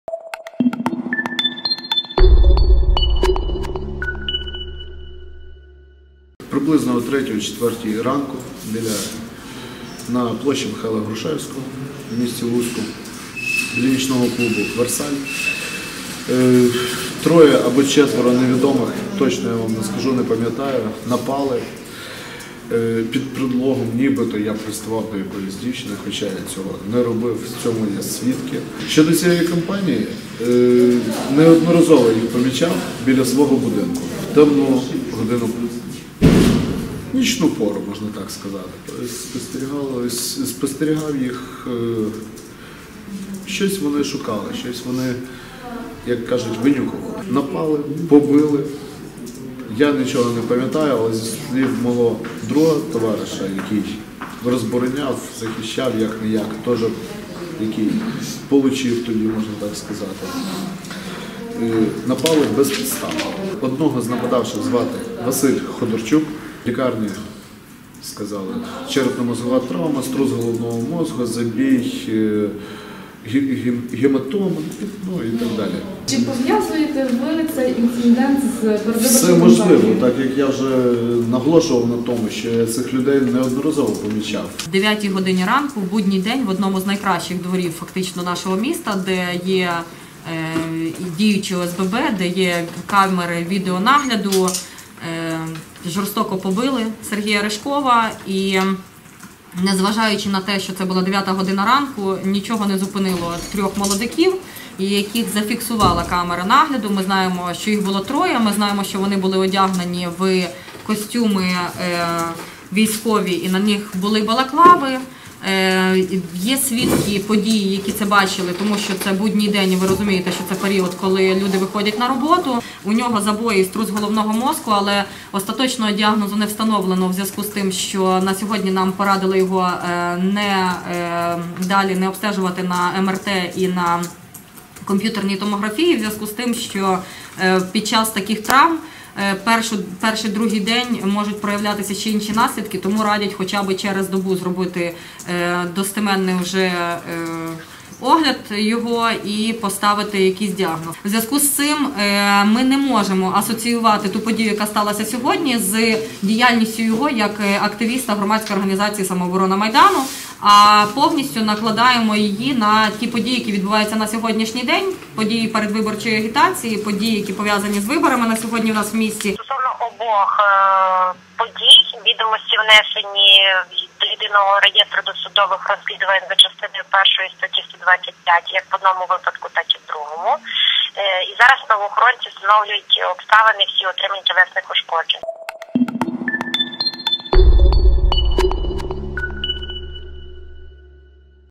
За добу у Луцьку побили двох кандидатів у депутати
З неділі, 11 жовтня, на понеділок, 12 жовтня, трапилося два випадки побиття кандидатів у депутати до Луцької міської ради. Сергія Вірного побили біля нічного клубу. На Сергія Рижкова напали у дворі його будинку. Нині правоохоронці розслідують факти вчинених правопорушень.